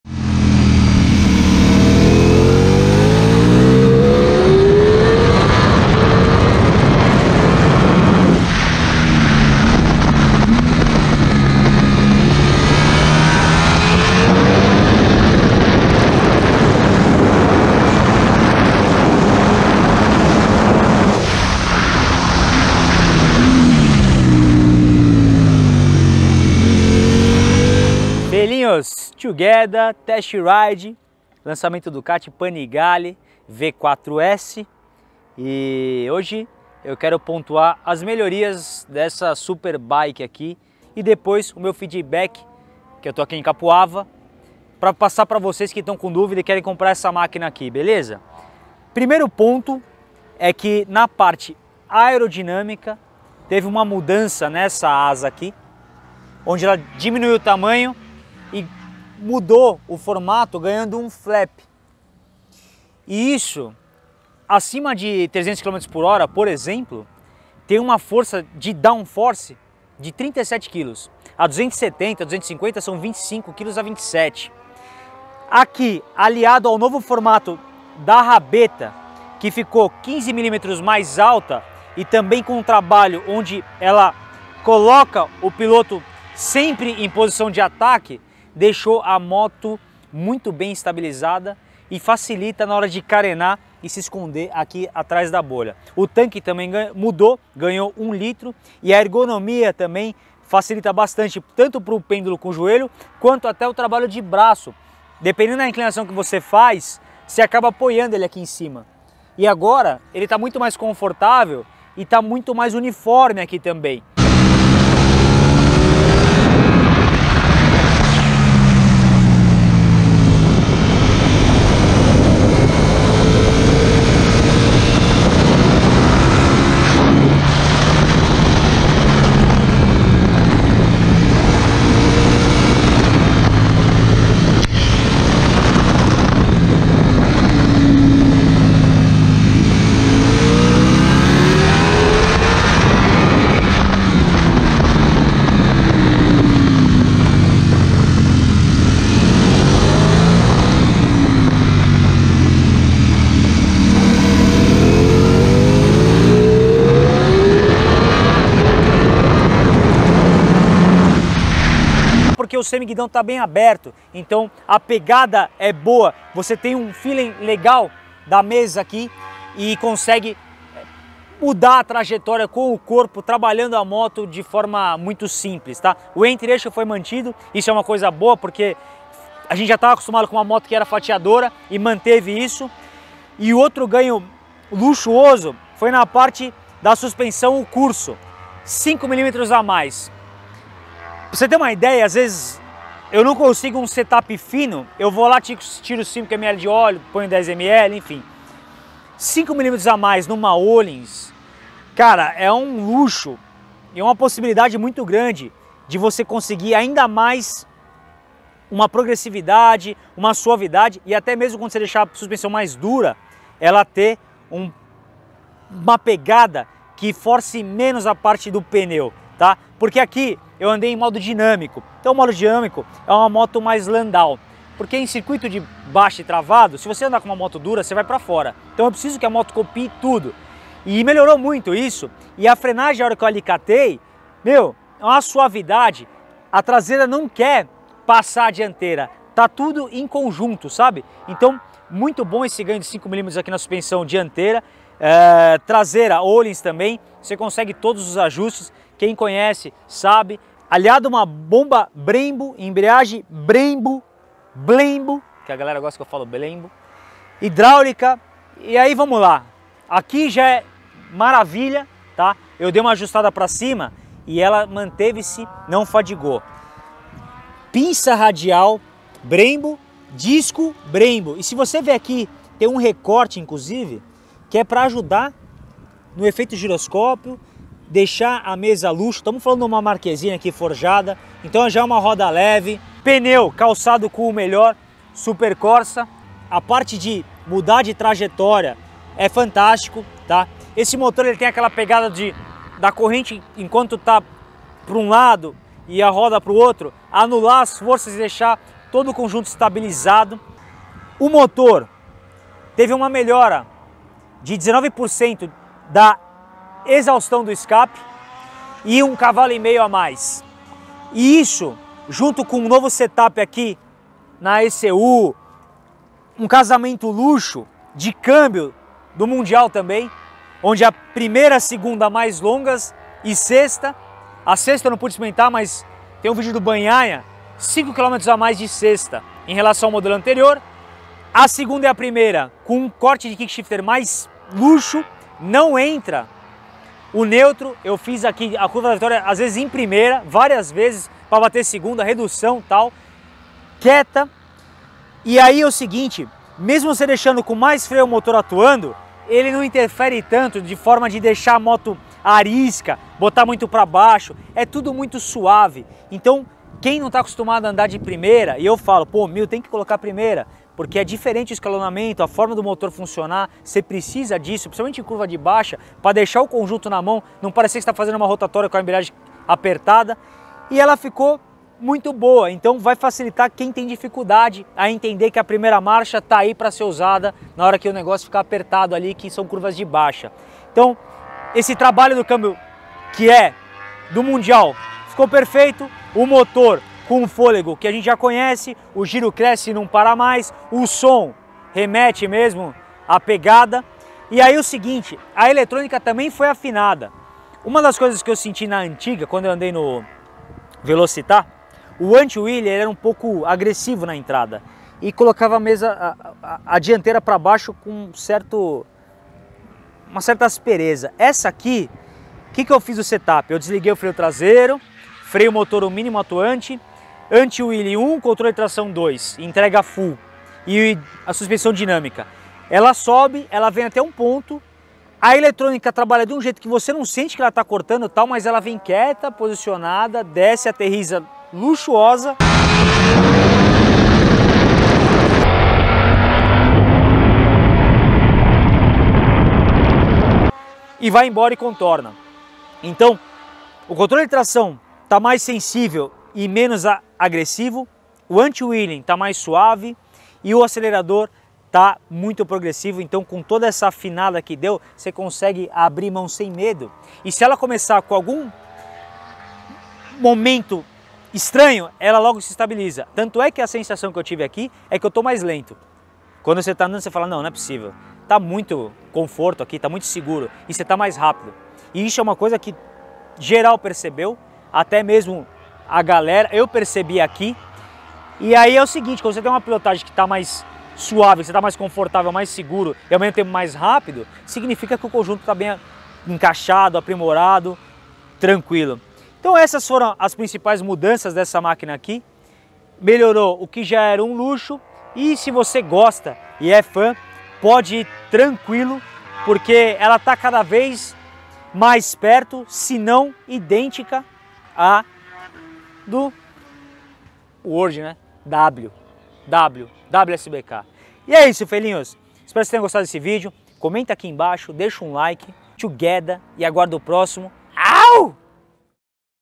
DUCATI PANIGALE V4 S 2023 | O QUE MUDOU EM RELAÇÃO AO MODELO ANTERIOR ? | TESTE RIDE COMPLETO
Feeelhos as mudanças que deixaram essa superbike ainda melhor.

Ficha técnica:
Motor: Desmosedici Stradale 90° V4, contrarrotativo, 4 válvulas por cilindro de acionamento desmodrômico, refrigerado a água..
Cilindrada: 1,103 cc.
Potência máx: 217 cv @13.000 rpm.
Torque máx: 126 Nm @9.500 rpm.
Transmissão: 6 marchas com Ducati Quick Shift (DQS) up/down EVO  2
Embreagem: antiblocante servo assistida de acionamento hidráulico e cilindro mestre autossangrável.
Chassi: em liga de alumínio.
Suspensão dianteira: Garfo Öhlins NIX30 de 43 mm totalmente ajustável com tratamento em nitreto de titânio. Öhlins Smart EC 2.0 semiativa com ajuste eletrônico de compressão e retorno.
Roda dianteira e traseira : Marchesini forjada em alumínio
Amortecedor traseiro: Unidade Öhlins TTX36 totalmente ajustável. Öhlins Smart EC 2.0 semiativa com compressão eletrônica e retorno. Balança monobraço em alumínio.
Pneu dianteiro: Pirelli Diablo Super Corsa SP 120/70 ZR 17.
Pneu traseiro: Pirelli Diablo Super Corsa SP 200/60 ZR 17.
Disco dianteiro: 2 discos semiflutuantes de 330 mm, pinças de 4 pistões M50 Brembo Monobloc Stylema® ancoradas radialmente e Bosch ABS Cornering. Cilindro mestre autossangrável.
Disco traseiro: Disco de 245 mm, pinça de 2 pistões com Bosch ABS Cornering.
Painel: digital de última geração com display colorido TFT de 5".
Peso seco: 174 kg.
Altura do assento: ajustável de 850 mm.
Entre eixos: 1,469 mm.
Capacidade do tanque: 17 L .